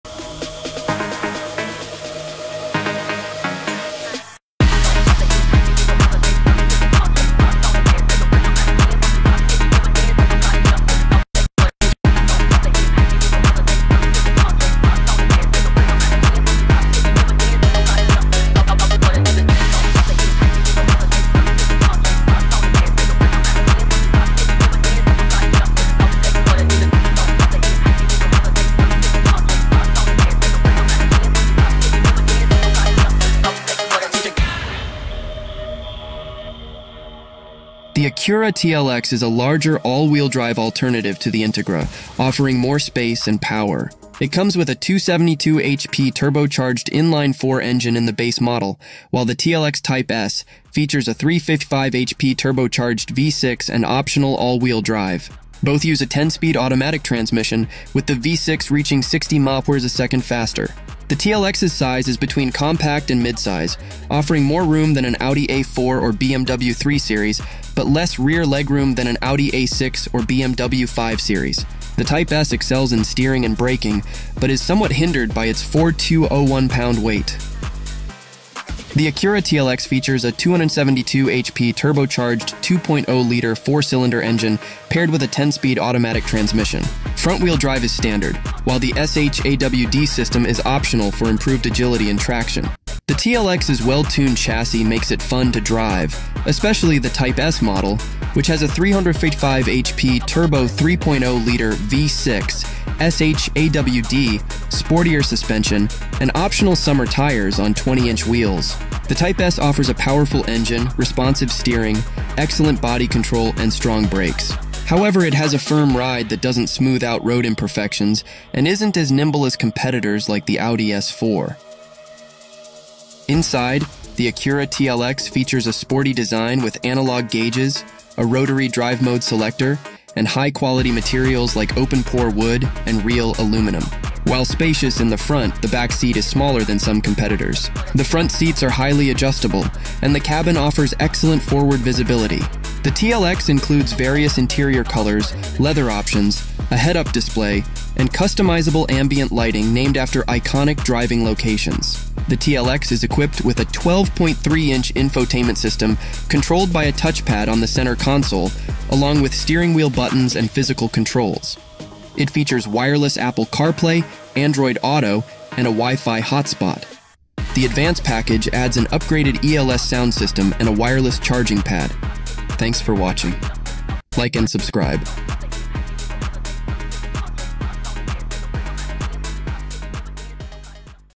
Acura TLX 2024: What You Need to Know
Welcome to our in-depth review of the 2024 Acura TLX! In this video, we explore everything you need to know about this impressive sedan, from its sleek design to its powerful performance and cutting-edge technology.

🚗 Exterior & Interior Design
Discover the sporty and luxurious design of the TLX, featuring stylish analog gauges, a rotary drive-mode selector, and premium materials like open-pore wood and real aluminum. We'll also highlight the spacious front seats, customizable ambient lighting, and the overall high-quality build of the interior.

🏁 Performance & Engine Options
We dive into the performance of the TLX, detailing the 272-hp turbocharged 2.0-liter four-cylinder engine and the more powerful 355-hp turbocharged 3.0-liter V-6 in the Type S model. Learn about the SH-AWD system that enhances agility and traction, and hear our thoughts on the driving experience.

📱 Technology & Features
Get a closer look at the 12.3-inch infotainment system, wireless Apple CarPlay and Android Auto, and the optional features available in the Advance package, such as the ELS sound system and wireless charging pad. We'll also discuss the intuitive touchpad controls and head-up display.

🛠️ Pros & Cons
We'll summarize the key strengths and weaknesses of the 2024 Acura TLX, including its firm ride, excellent build quality, and how it compares to competitors in terms of rear passenger space and nimbleness.

Don't miss our test drive impressions and final verdict on whether the 2024 Acura TLX is the right sedan for you!

Music from Uppbeat (free for Creators!)